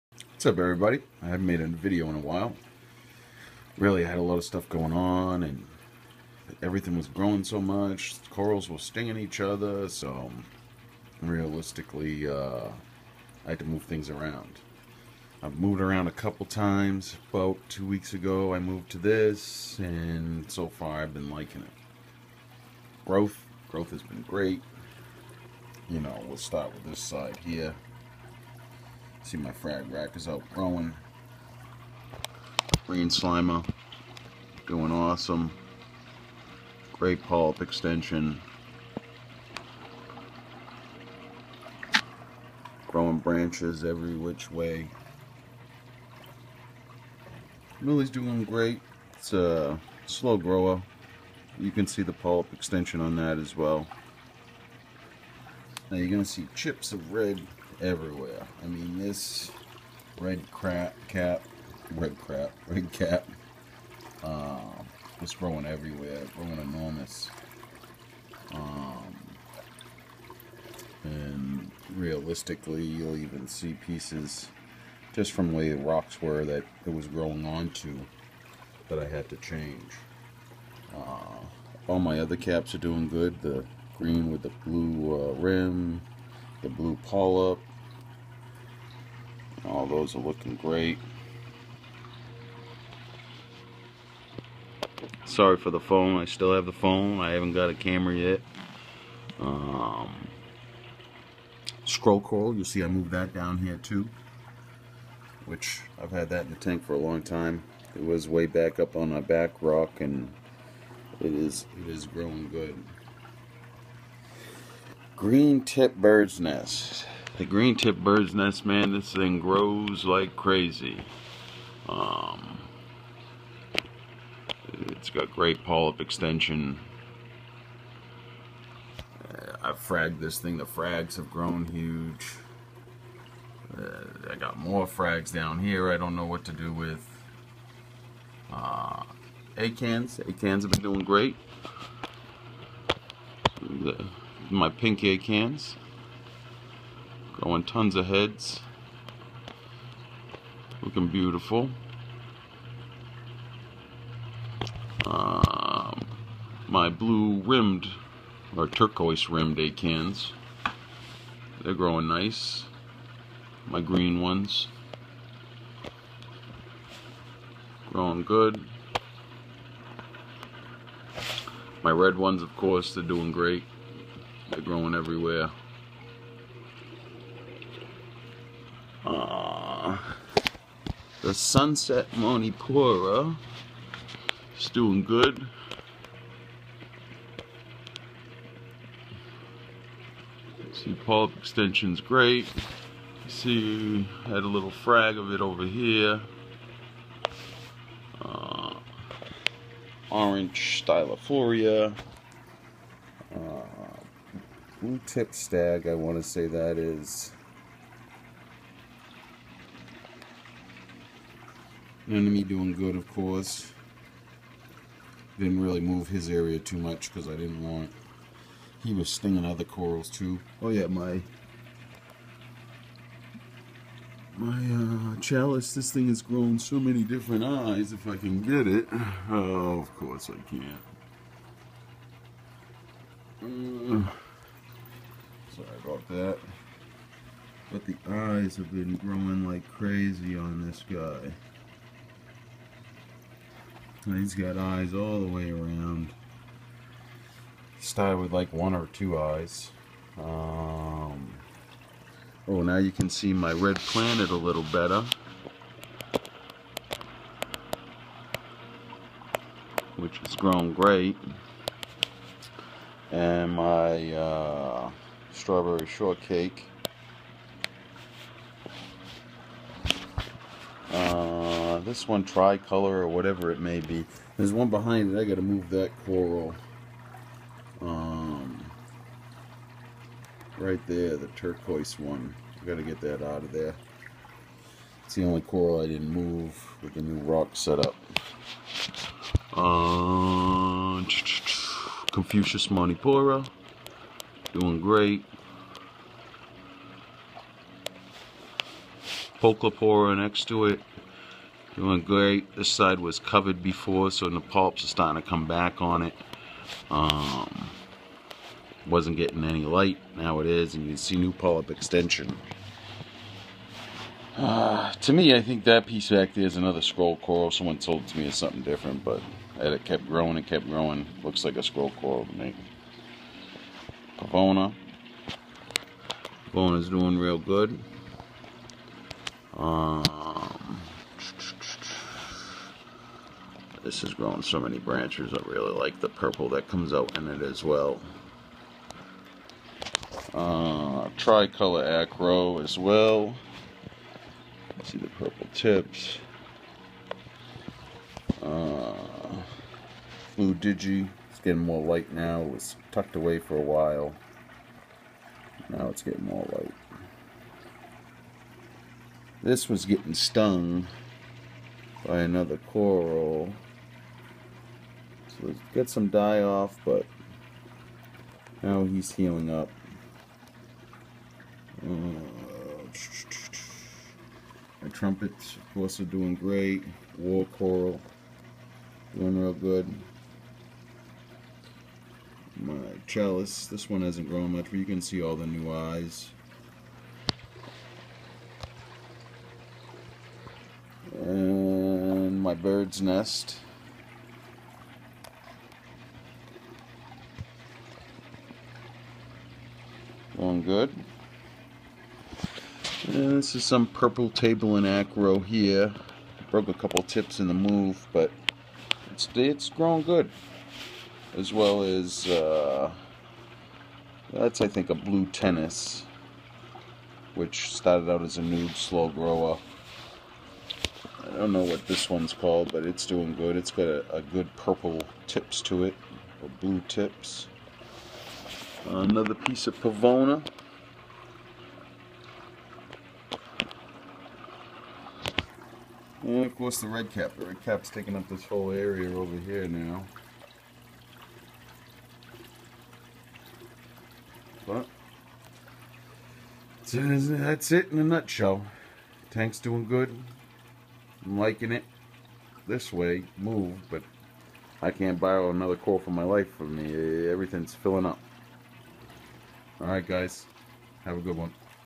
Update salt water reef growth
Had to make changes everything was growing into each other